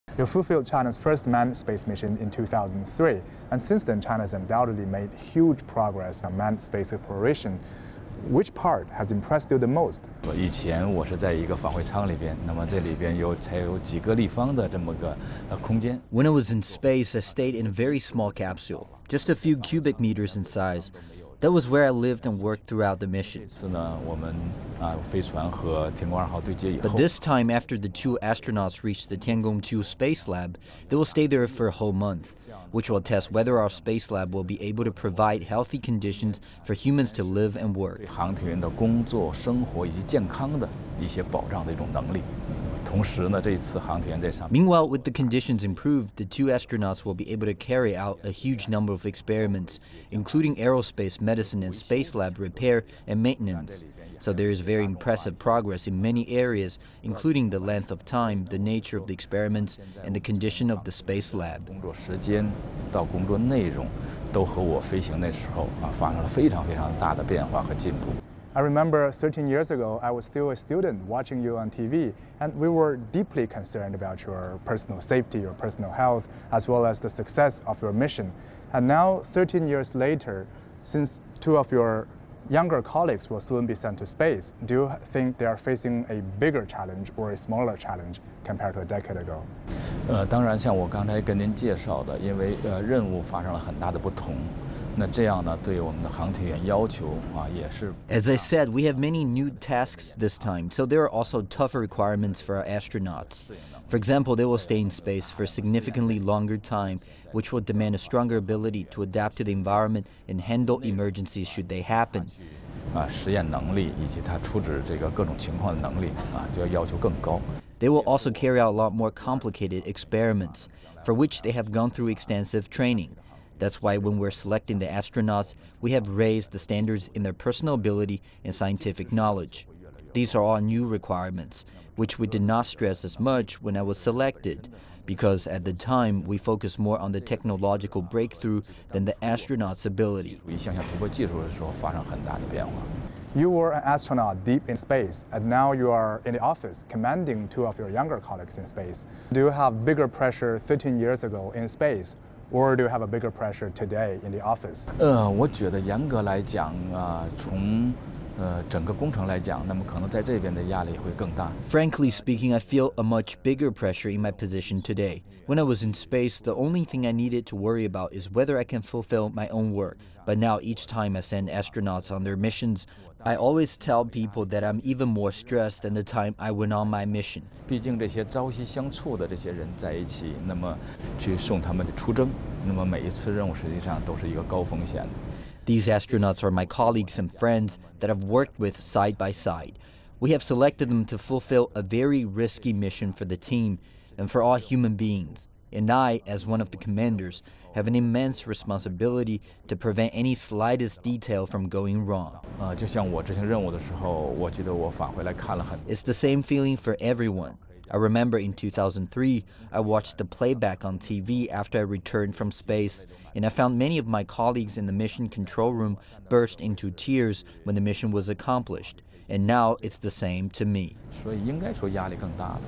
Exclusive interview with China’s first astronaut Yang Liwei
Yang Liwei, China's first astronaut, has told CCTVNEWS that he feels more pressure in the control room than in space. Yang, one of the commanders of the Tiangong-2 mission, also shared his views on that program ahead of Thursday evening's launch.